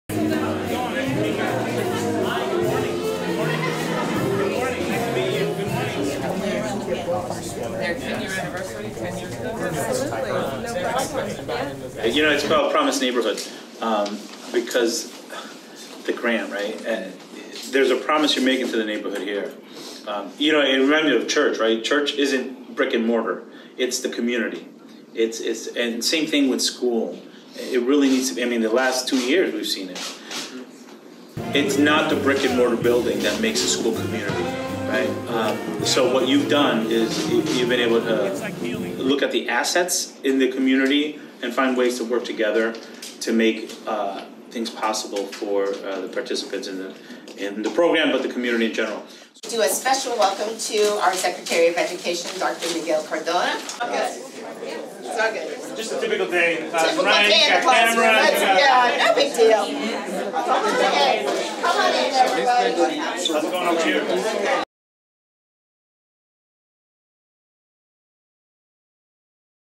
U.S. Secretary of Education Miguel Cardona visits Broward College
U.S. Secretary of Education Miguel Cardona and Rep. Debbie Wasserman Schultz participated in a conversation with Broward College students on April 4, 2022. They talked about educational opportunities and workforce training.

Video by Carl Juste / Miami Herald